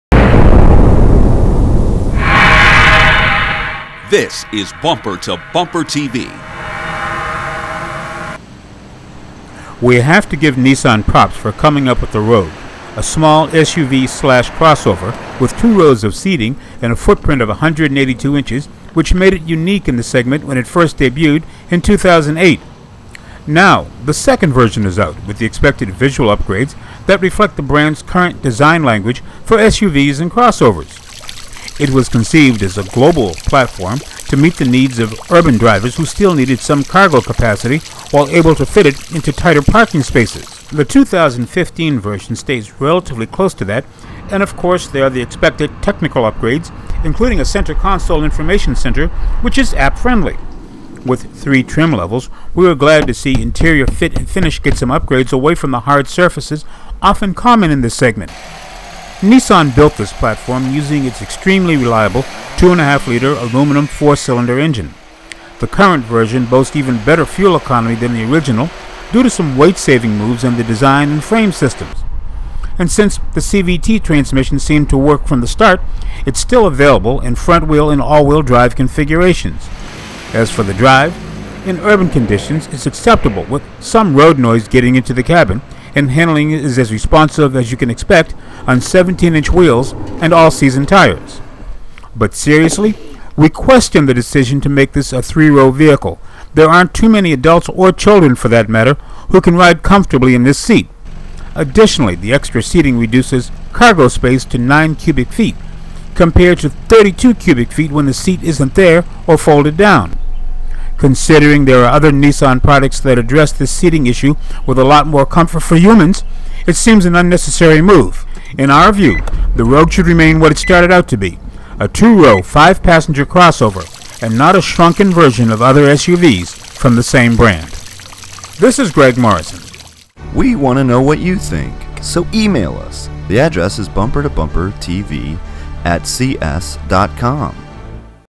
Is the Nissan Rogue getting a little too big for its own good?
A reality of the crossover or small SUV segment these days is that often smaller is better. Bumper2Bumpertv has a look at the current version of the Nissan Rogue which is a long time offering but may have gone a little off track.